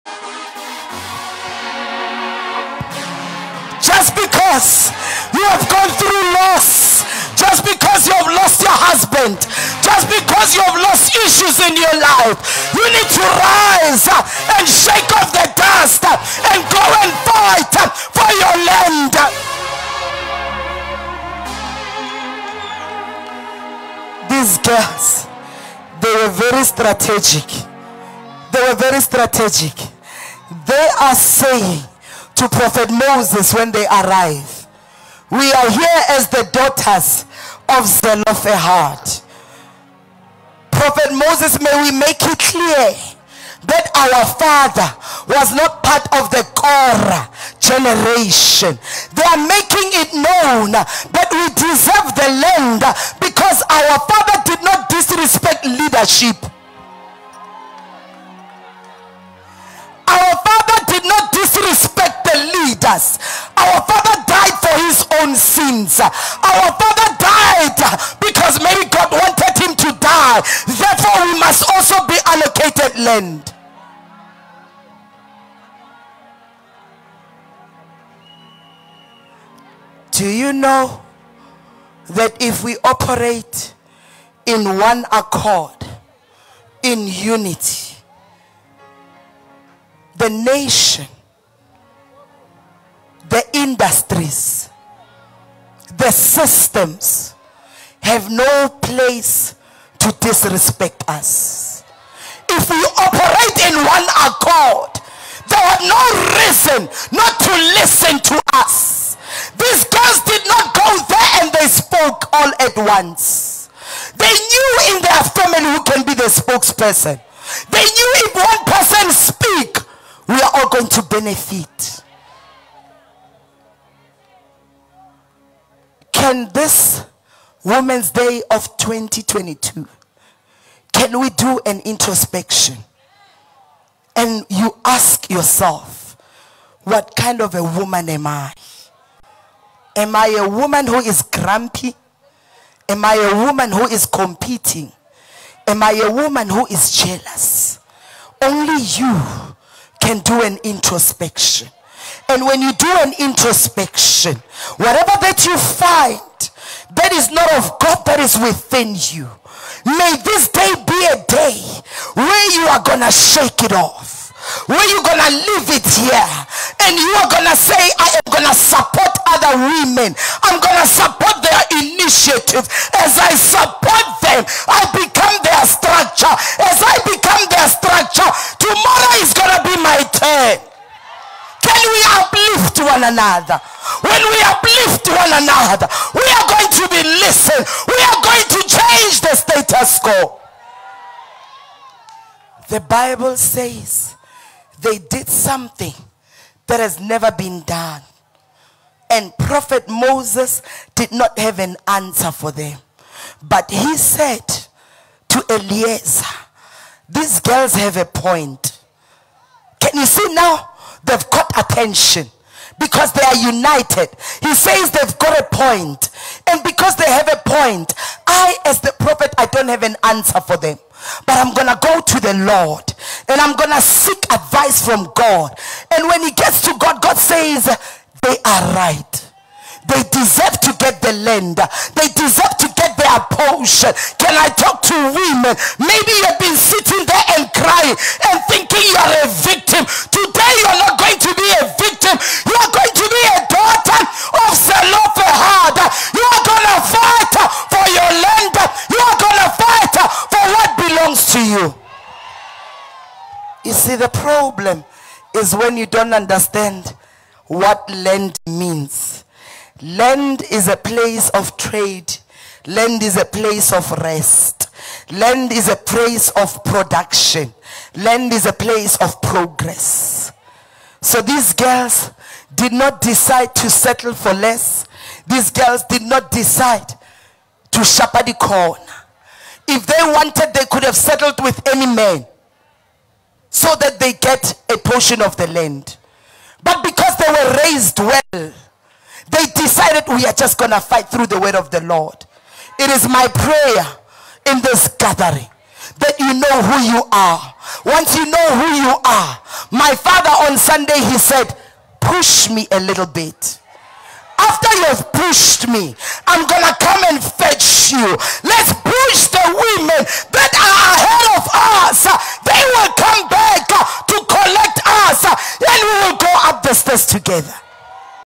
Pastor Nomonde - Let Us All Operate In One Accord & Uplift One Another
Prophetess Nomonde Ndlovukazi - Fight For What Belongs To You Till The Very End Women Of Valour.
Fight For Your Land.

Support Other Women!!!